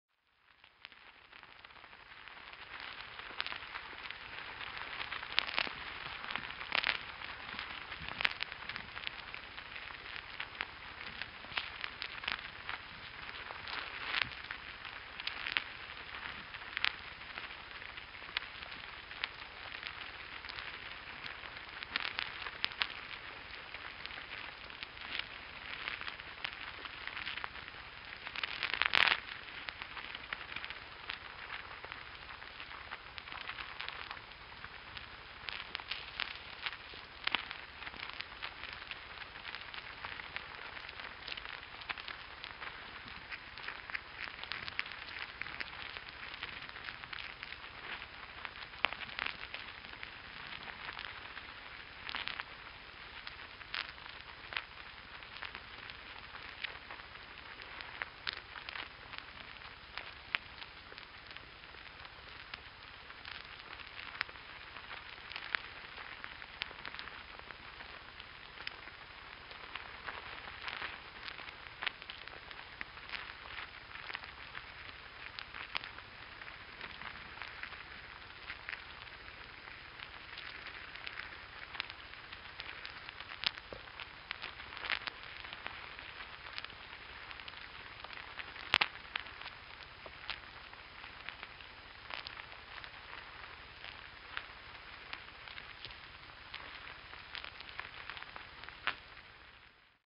Ant Hill Activity, piezo contact microphone recording
From the Location Sound Films 2006 series. Two contact microphones were stuck into the top of an ant hill. What you hear is the sound of the ants crawling across the surface of the microphones. Similar material went into my film 'Sound Aspects of Material Elements'